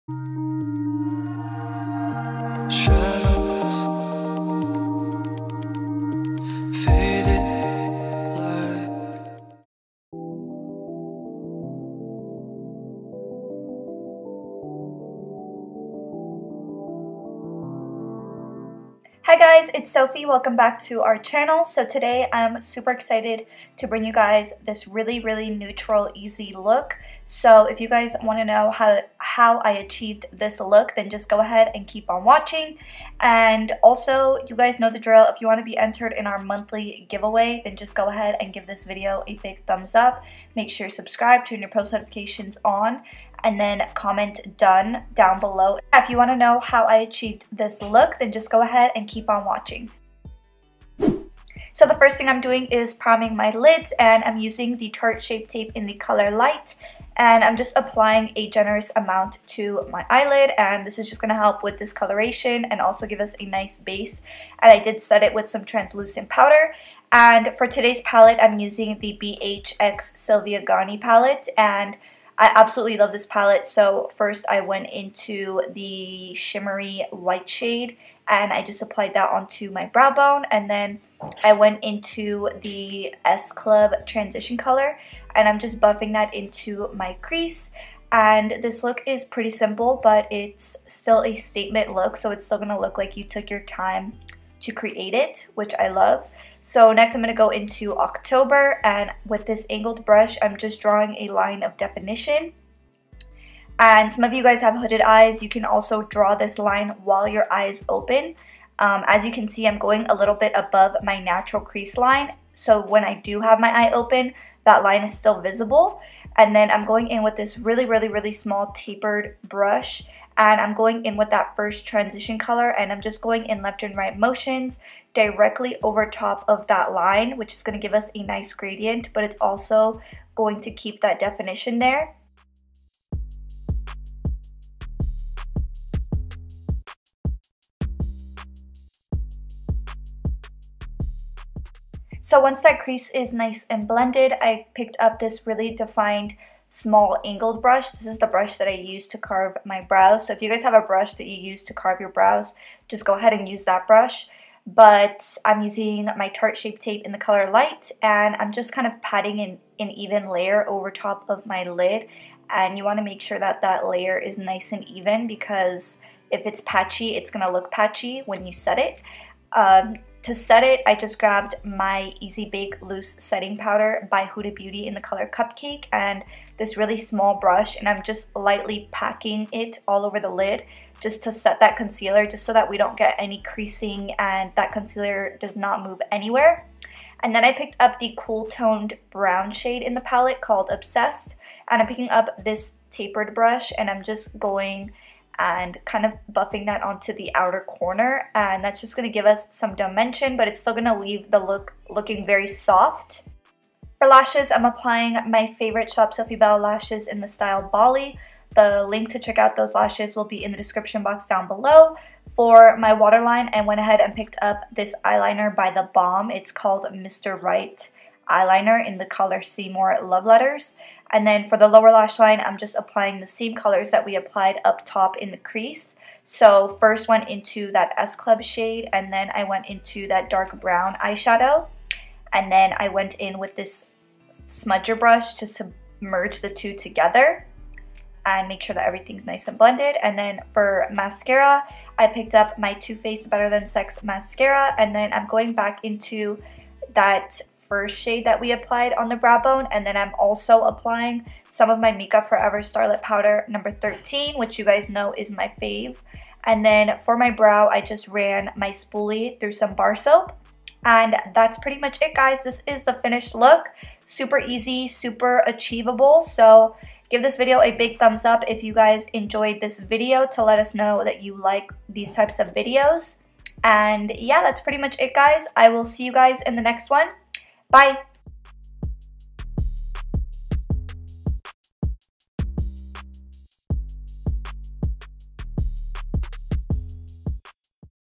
EVERYDAY MAKEUP EYE LOOK TUTORIAL
SHOP OUR LASHES↓⇣↓⇣↓
All Styles are Cruelty-Free + Faux Mink

 ↓⇣↓⇣↓PRODUCTS MENTIONED ↓⇣↓⇣↓
Makeup Details ⇣
Lashes: @shopsofiebella Bali Lashes
Brows: @anastasiabeverlyhills brow powder duo “Dark Brown” & Clear Brow Gel
Eyes: @bhcosmetics x @sylviagani Palettek “S-Club” , “October” in the crease, ”Prince ” in the inner corner & on the brow bone, “bear” on the lower lash line;
Mascara: @toofaced better than sex mascara
@tartecosmetics fair shape tape concealer to prime the lid
Makupforever Starlit powder #13 On brow bone & inner corner
@thebalm Mr.write liner “Seymour Romance in my waterline